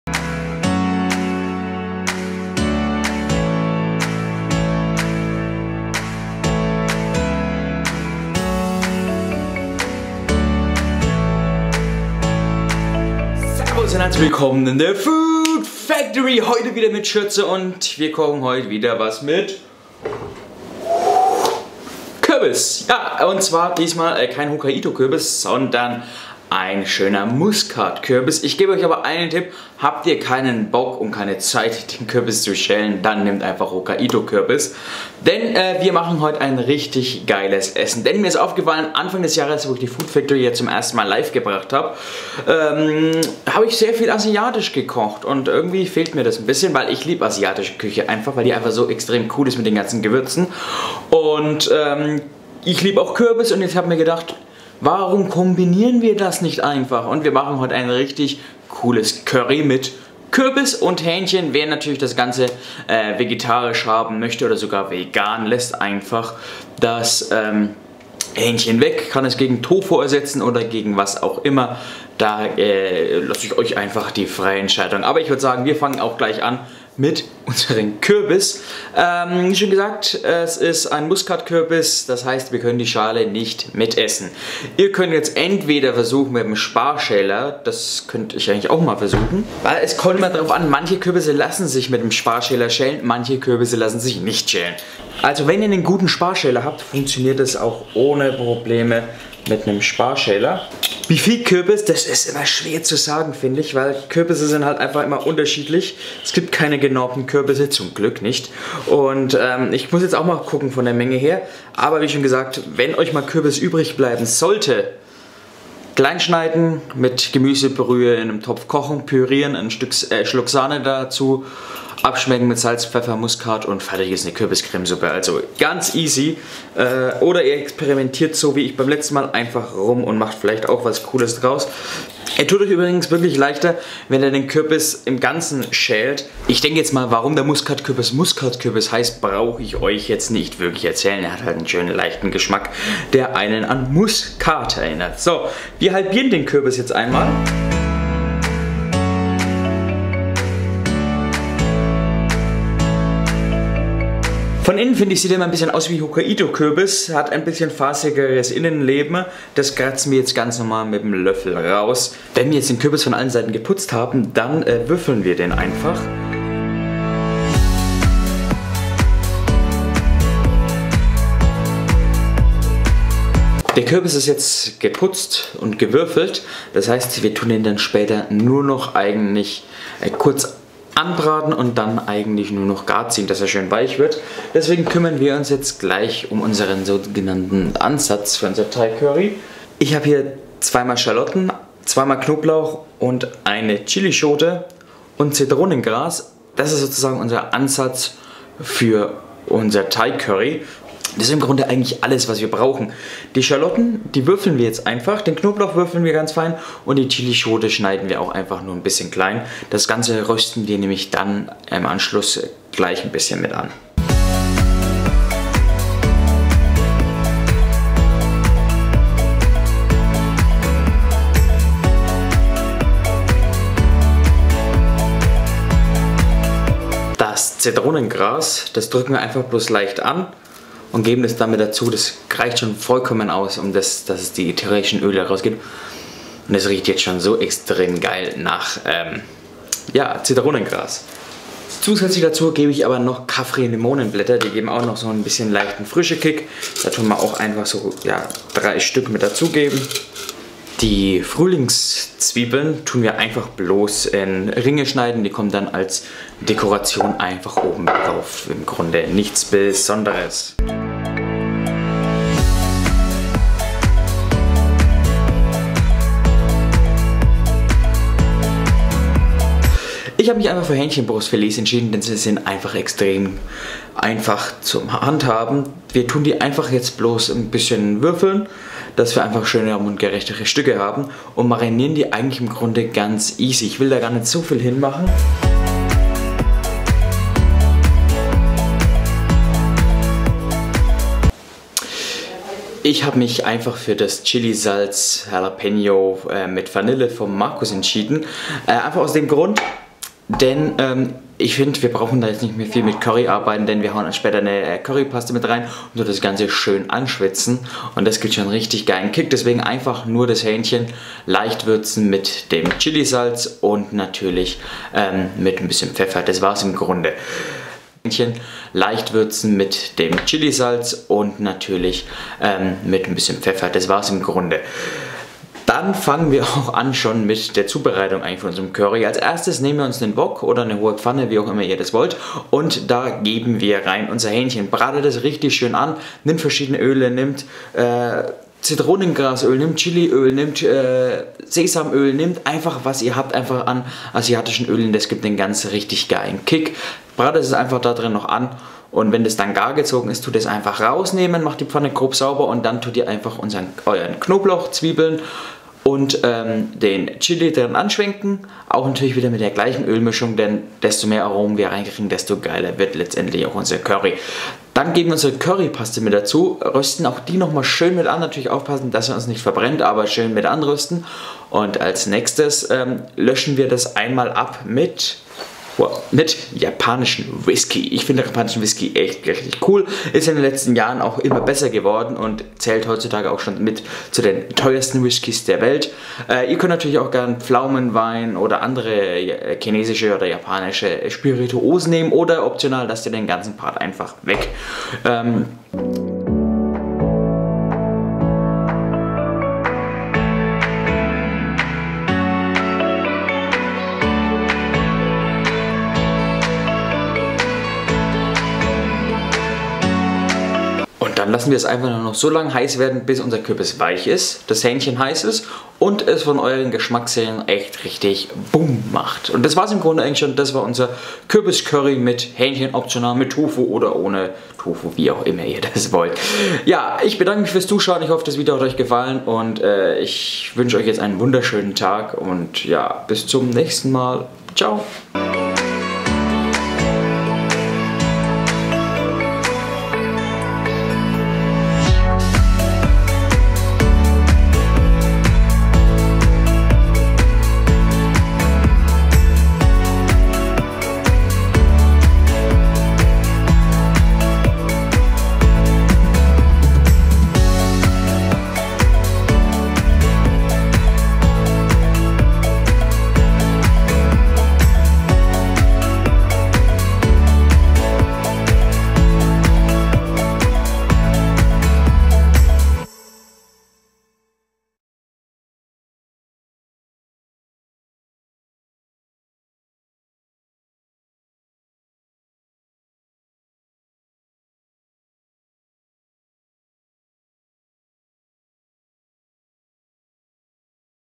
Kürbis Curry mit Hähnchen | Food Factory
Kürbis Curry mit Hähnchen

Es giebt doch nichts schöneres als ein gutes und feuriges Curry, doch geht das auch mit Kürbis? Ja natürlich und es ist mega geil.

300 Gramm  Muskatkürbis
300 Gramm  Hähnchenbrust Filet
2   Schalotten
2   Zehen Knoblauch
1 -2 Esslöffel   Currypaste (Nehmt da einfach was euch am besten Schmeckt)
200 Milliliter   Kokosmilch
1 Stange   Zitronengras
2 -3 Blätter   kaffir Limettenblätter
1   Chili Schote
   Chiliöl, Zitronengrasöl oder andere Öle
   Kürbiskerne
   Salz & Pfeffer
   Chili - Jalapeno - Salz mit Vanille von Markus Kieslich
10 Milliliter Japanischer Whisky

Wer möchte kann gerne noch mehr Gemüse dazugeben.

So fängst du an:

Schritt 1.

Kürbis schälen und das Kerngehäuse entfernen. Den Kürbis in mundgerechte Stücke schneiden.

Schritt 2.

Knoblauch und Zwiebeln schälen. Die Zwiebeln, Knoblauch und die Chili fein würfeln.

Schritt 3.

Die Hähnchenbrust waschen und trockentupfen. In mundgerechte Stücke würfeln. Das Fleisch würzen.

So wird gekocht:

Schritt 4.

In einer hohen Pfanne die Öle erhitzen und das Fleisch darin anbraten. Wenn das Fleisch von allen Seiten schön angebraten ist, aus der Pfanne nehmen.

Schritt 5.

Die Pfanne sauber machen und wieder Öl hineingeben. Darin Knoblauch, Zwiebeln und die Chilis anschwitzen. Den Kürbis dazugeben und auch mit anbraten.

Schritt 6.

Die Currypaste dazugeben und mit anrösten, mit japanischem Whisky ablöschen. Das Fleisch wieder dazugeben und mit etwas Hühnerbrühe und der Kokosmilch aufgießen.

Die Limettenblätter dazugeben und das Zitronengras andrücken und mit in die Pfanne geben,

Schritt 7.

Das ganze so lange köchelt lassen, bis der Kürbis weich ist. Noch mal abschmecken und dann servieren.

In eine bowl Füllen und mit Kürbiskernen und Frühlingszwieblen servieren.